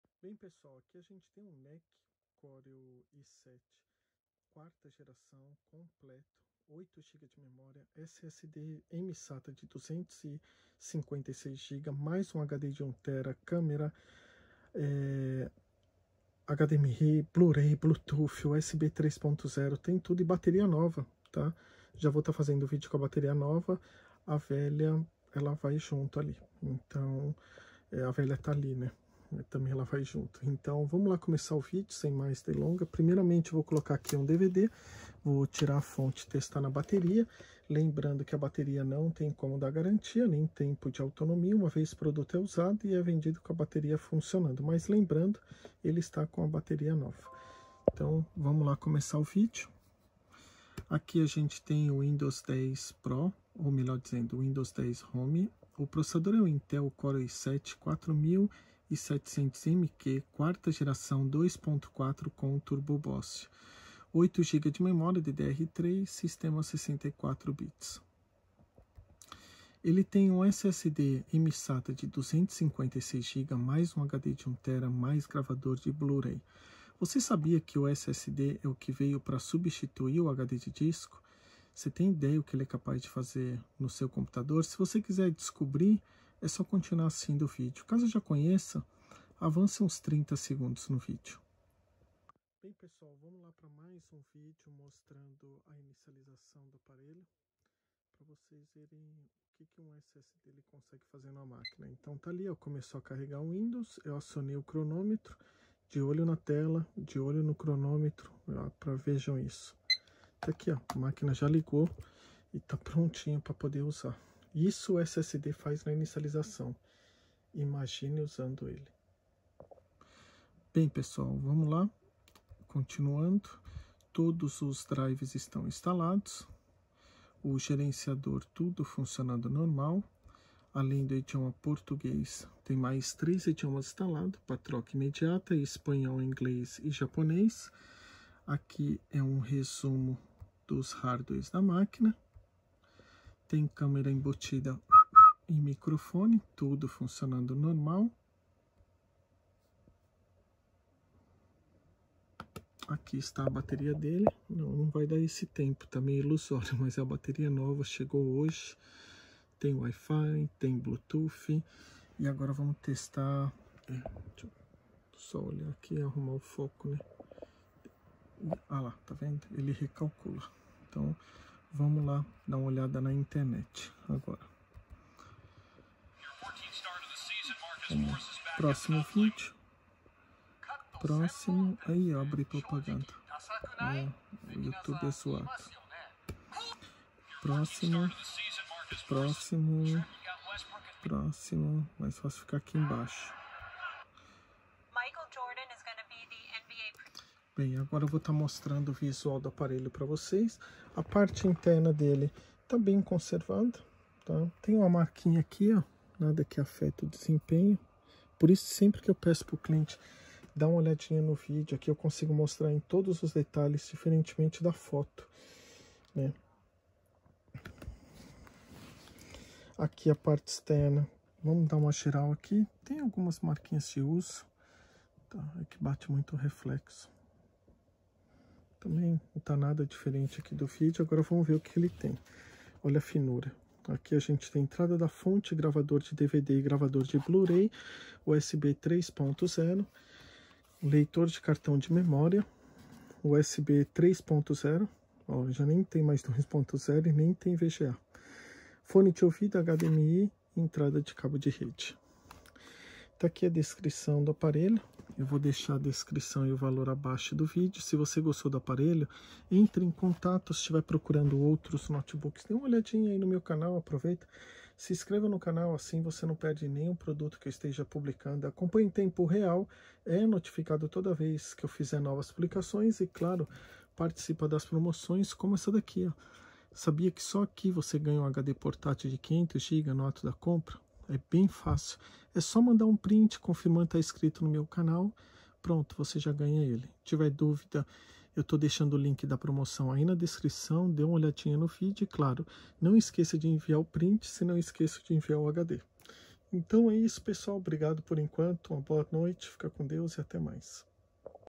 Vendido🎉Notebook Nec Core i7 Completo 8Giga Mem SSD256G + HDD 1Tera HDMI WebCam USB3.0 Blu-ray 🙀
¥30,000

Modelo 2014
Intel Core I7-4700MQ  CPU   2.40GHz  ~  3.40GHz    Turbo Bosst      (4 Geração )
Windows 10 Home 64bits
SSD MSata 250 Giga  + HDD 1 Tera
8 Giga Memoria DDR3
Vídeo Intel HD Graphics 4600
Bateria Funcionando Normal (3 Células Nova)
Tela 15.6 Polegadas Led Brilhante
Wi Fi
Bluetooth
WebCam Embutida
Gravador de DVD e Gravador de Blu-ray

Para Adiquirir seu HDD Externo Grátis veja o vídeo

Envio para pagamento na entrega de ¥1,500~¥2000 independente da região!
Somente envio dentro do Japão 🙏